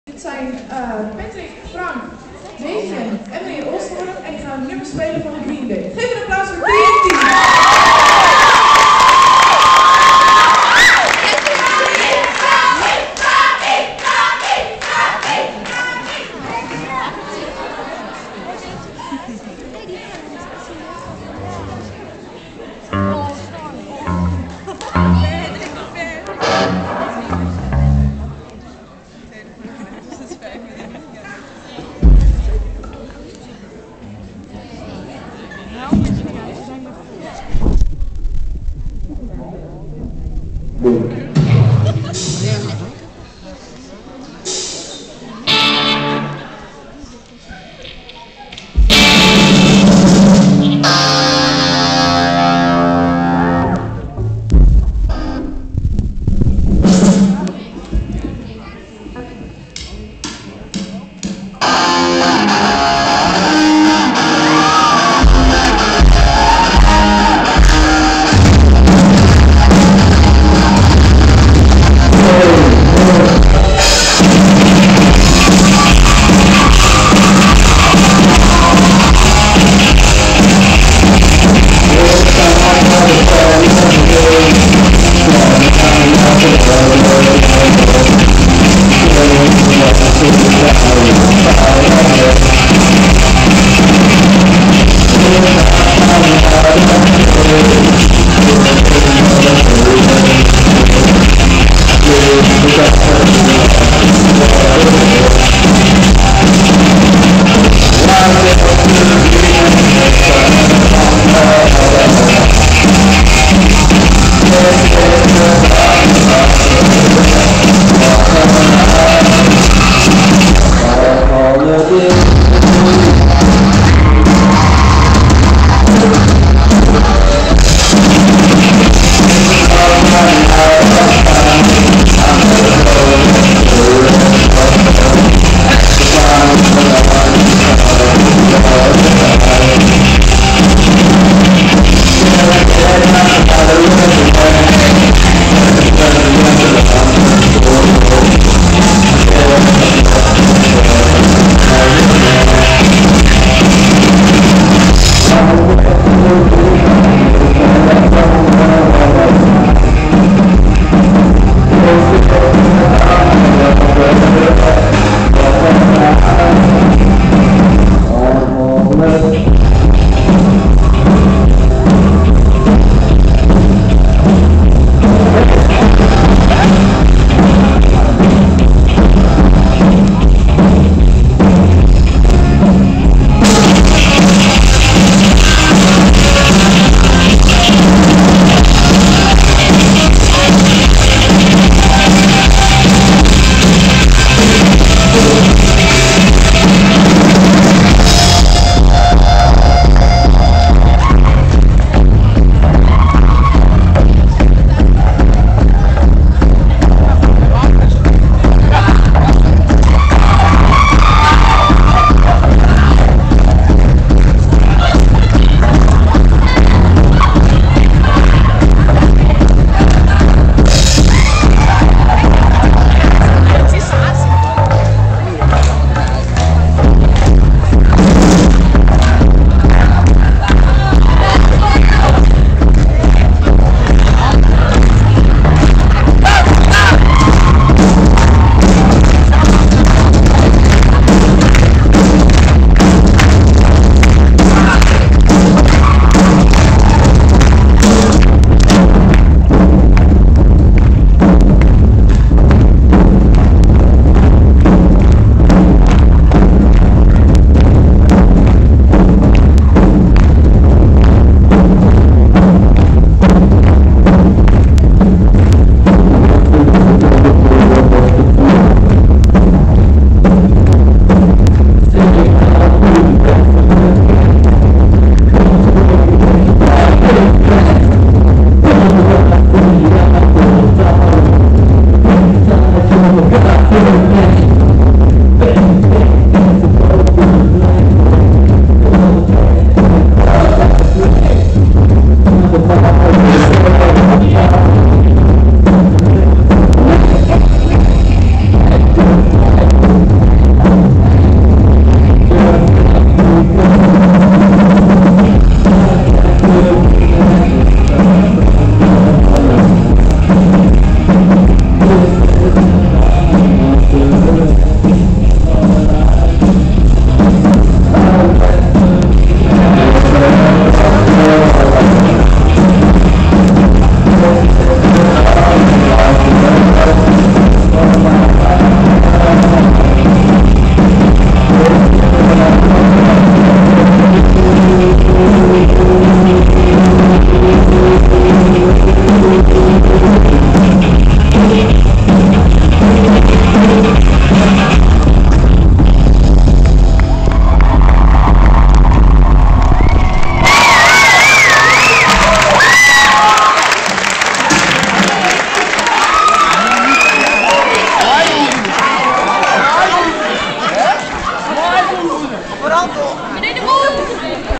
TNT gig op de Gaudí Night in De Purmaryn, ROCKON!!!
Check voor een mooie beschrijving van de voorbereiding en het optreden

Excuses voor de kwaliteit, tis maar een iphone 3G. Valt misschien nog wel wat aan te doen. Iemand suggesties?